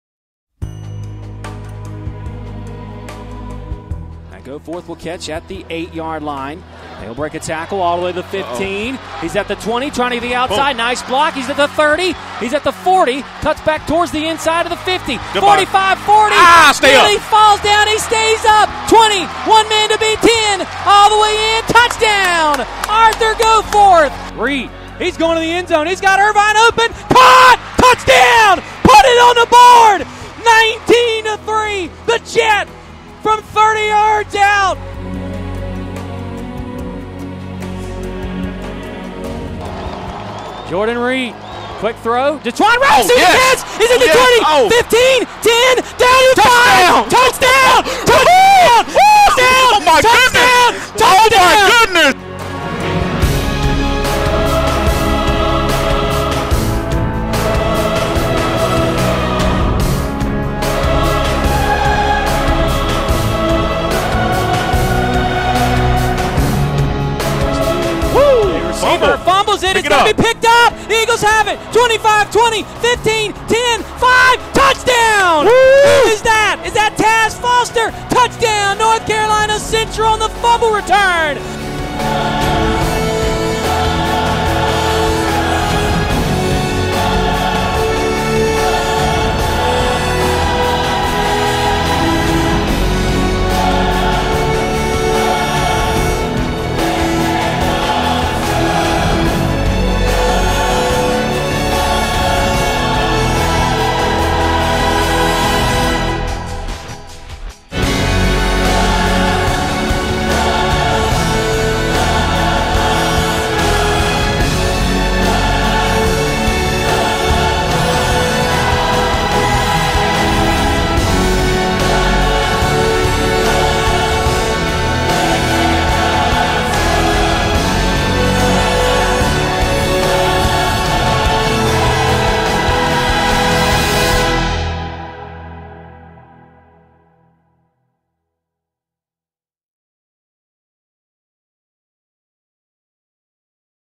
2013 NCCU Football Teaser
The 2013 NCCU Football season is just around the corner!  Get your season tickets now by and reserve your seat for six home games as the maroon and gray look to make a run to its first title since re-joining the MEAC!